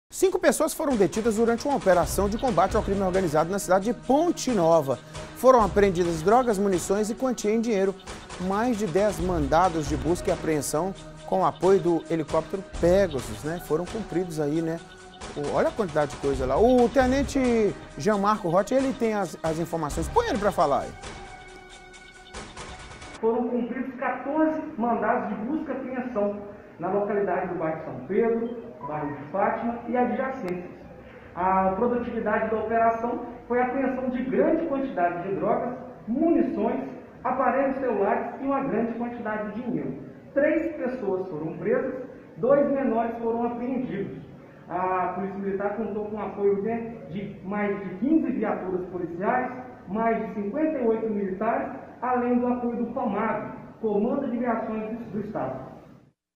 Ponte Nova: operação "Impacto" prende suspeitos de tráfico e apreende drogas e munições
Cinco pessoas foram detidas durante uma operação de combate ao crime organizado na cidade de ponte nova. Foram apreendidas drogas, munições e quantia em dinheiro. Mais de 10 mandados de busca e apreensão com o apoio do helicóptero Pégasus foram cumpridos. O tenente Jean Marco Hott tem as informações.

Jean Marco Hott
Tenente da Polícia Militar

Imagens cedidas
Polícia Militar